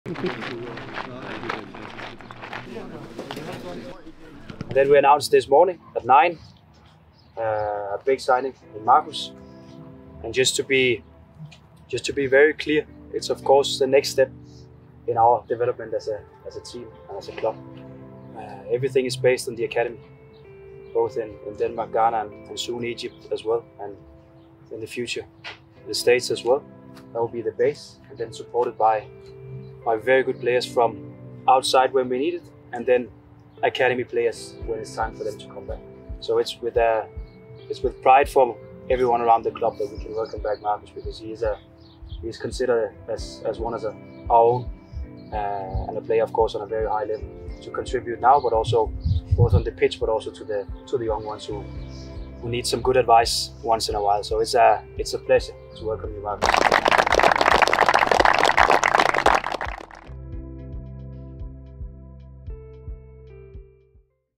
Ingvartsens første træning efter hjemkomsten: Næste skridt i klubbens udvikling
Marcus Ingvartsen blev taget godt imod af spillere og stab, da han var med til dagens træning i Østrig ❤️💛

ID: MB01NRXMBUKZLCK